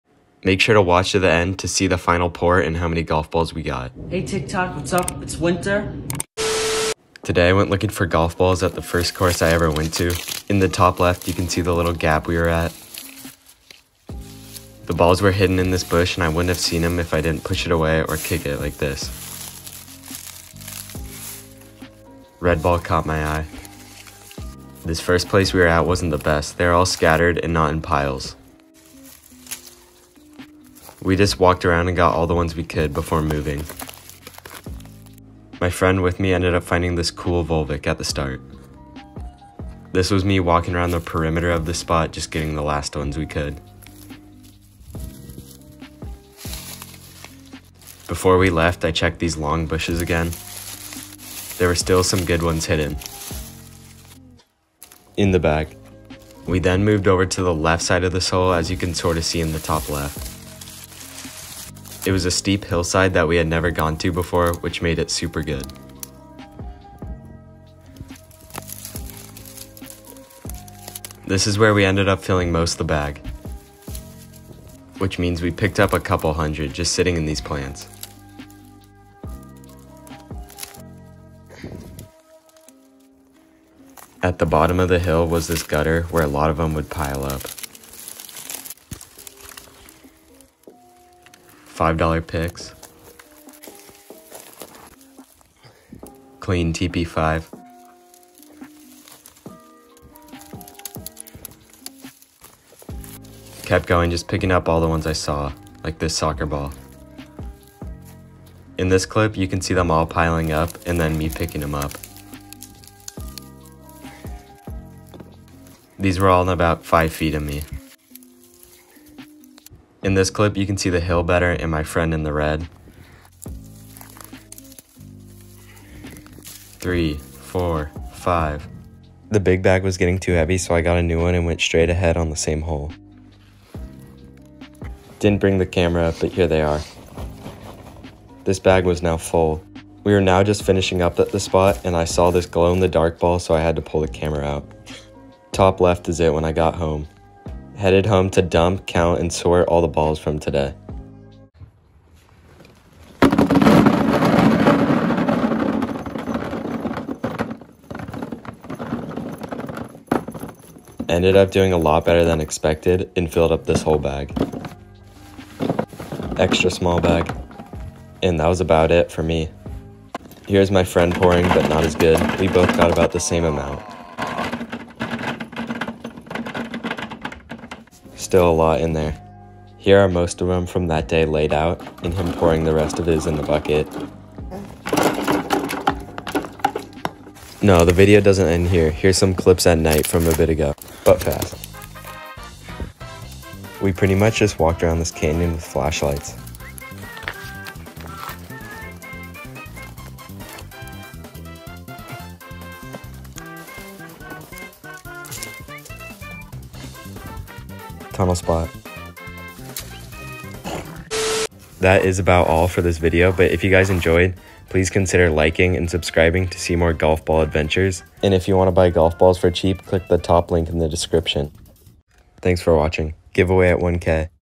Finding Thousands of Lost Golf Balls In The Woods
Visit here to buy golf balls for cheap!